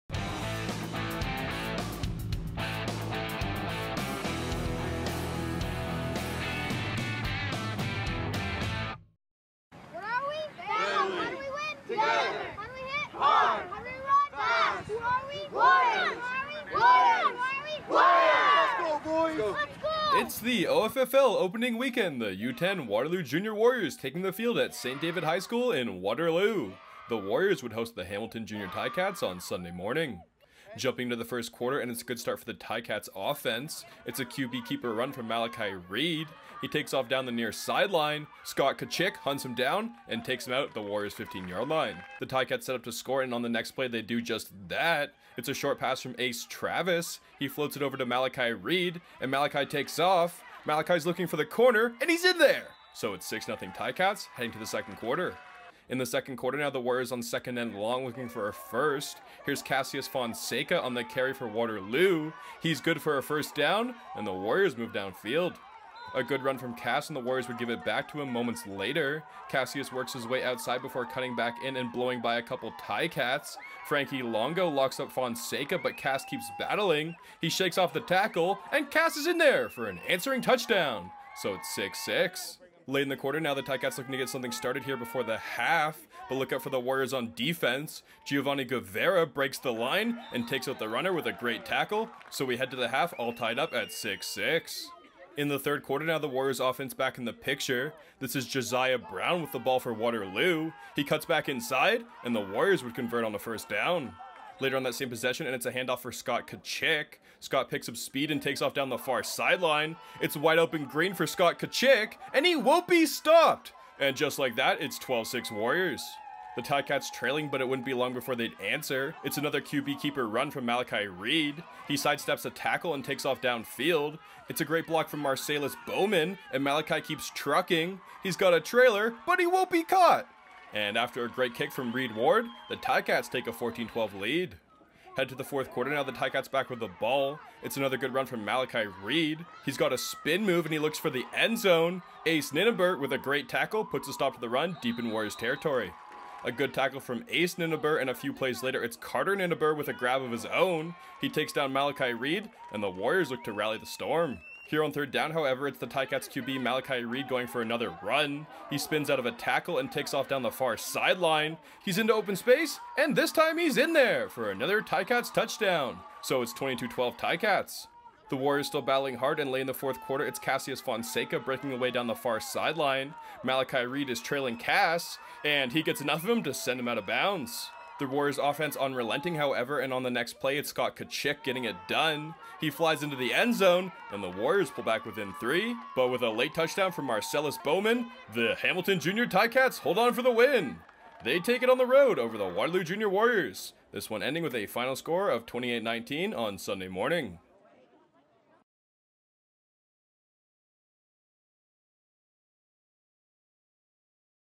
OFFL Football (Under-10) - Hamilton Jr. Tiger-Cats vs Waterloo Jr. Warriors
Highlights from Sunday September 17th between the Jr. Ticats and Jr. Warriors at St. David high school in Waterloo.

This video is presented by Waterloo Region Minor Football. Learn more about their tackle and flag programs by visiting